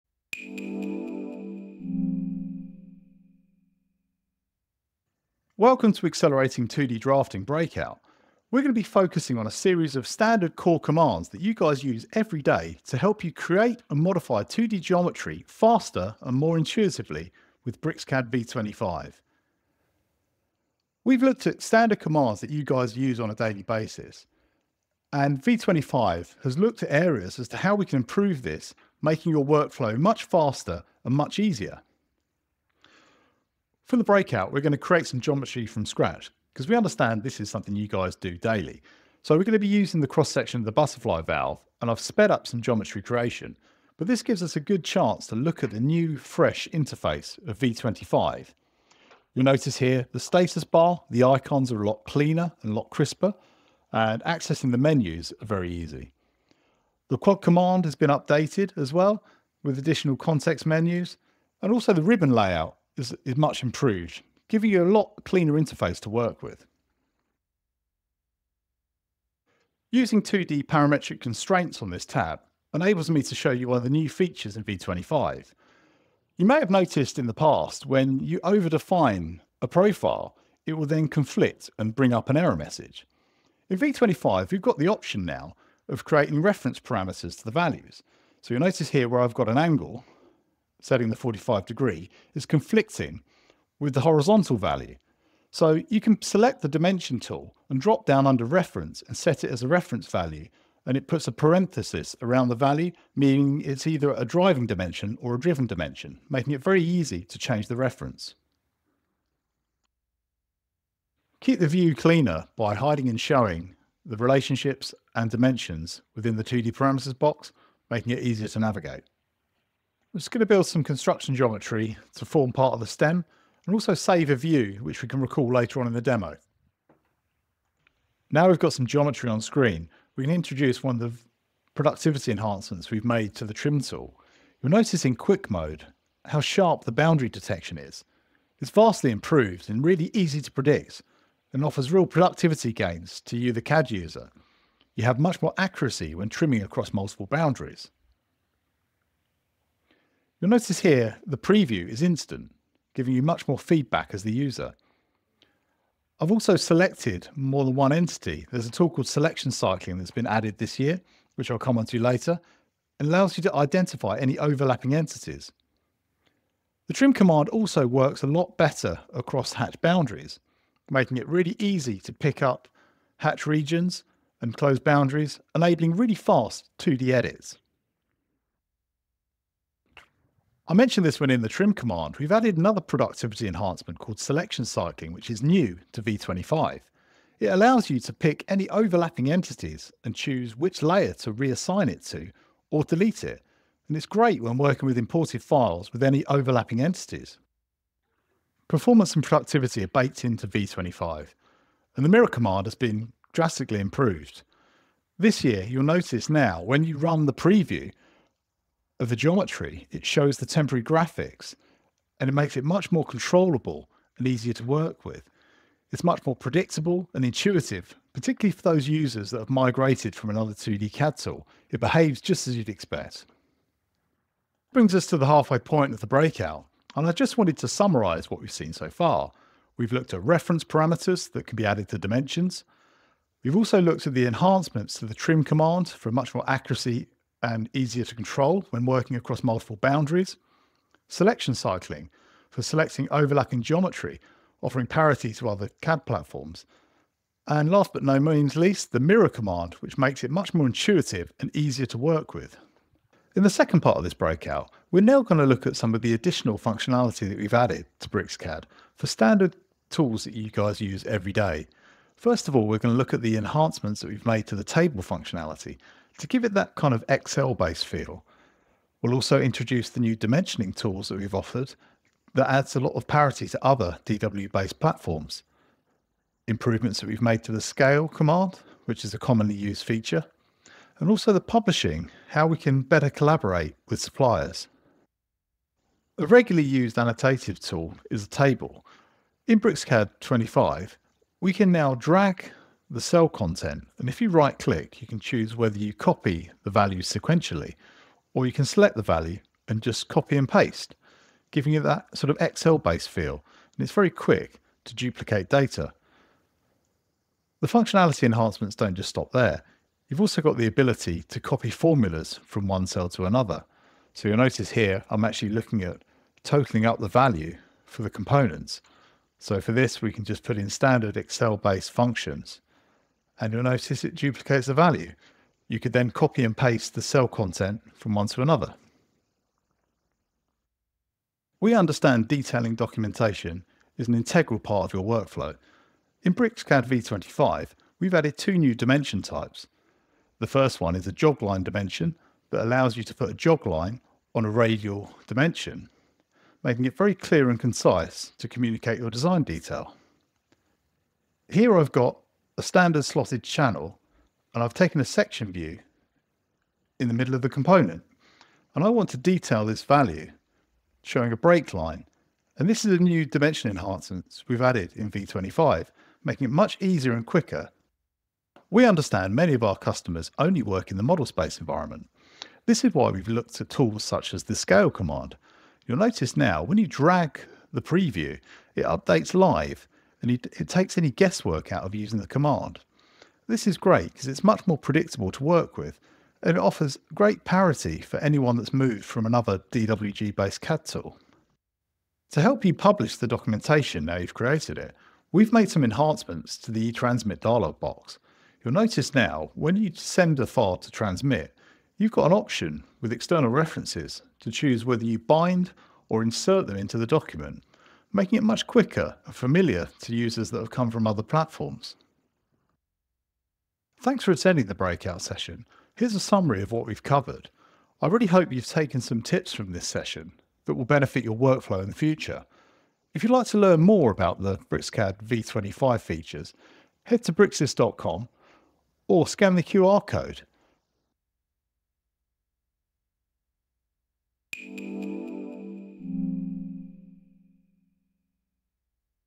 Accelerating 2D Drafting with BricsCAD® Lite and Pro V25 │Breakout Session
Learn how to complete common 2D drafting tasks quickly with new and enhanced features to make 2D geometry smarter and easier.

Upgrade to BricsCAD V25 today!